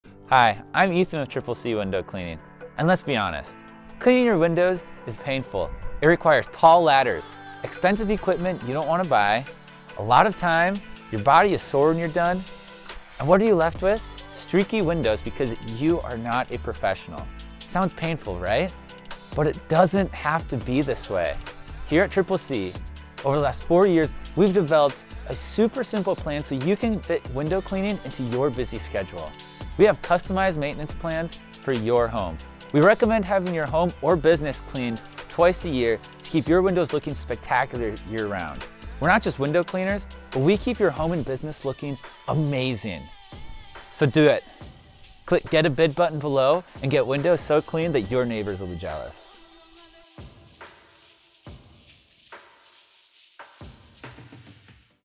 TripleC Window Cleaning
Need your windows cleaned? TripleC has you covered. I had a great time shooting this promotional video for them!